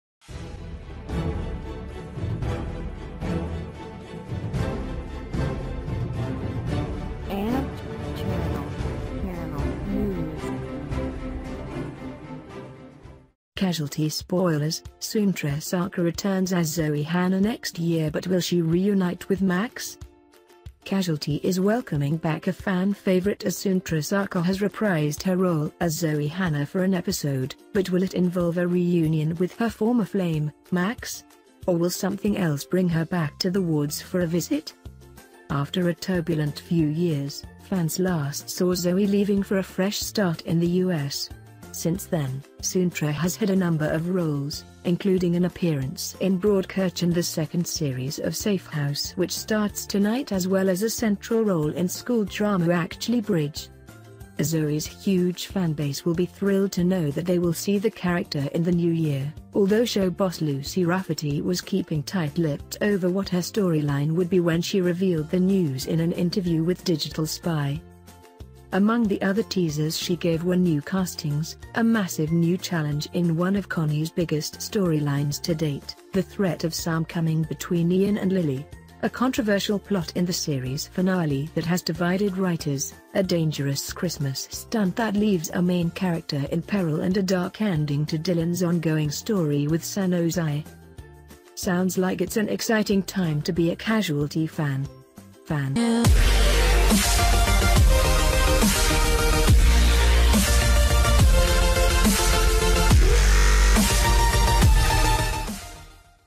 Casualty spoilers: Sunetra Sarker returns as Zoe Hanna next year but will she reunite with Max?
Casualty is welcoming back a fan favourite as Sunetra Sarker has reprised her role as Zoe Hanna for an episode – but will it involve a reunion with her former flame, Max? Or will something else bring her back to the wards for a visit?
After a turbulent few years, fans last saw Zoe leaving for a fresh start in the US. Since then, Sunetra has had a number of roles, including an appearance in Broadchurch and the second series of Safe House which starts tonight as well as a central role in school drama Acjley Bridge.
Zoe’s huge fanbase will be thrilled to know that they will see the character in the new year, although show boss Lucy Raffety was keeping tight lipped over what her storyline would be when she revealed the news in an interview with Digital Spy.

Among the other teasers she gave were new castings, a massive new challenge in one of Connie’s biggest storylines to date, the threat of Sam coming between Iain and Lily, a controversial plot in the series finale that has divided writers, a dangerous Christmas stunt that leaves a main character in peril and a dark ending to Dylan’s ongoing story with Sanosi.
Sounds like it’s an exciting time to be a Casualty fan!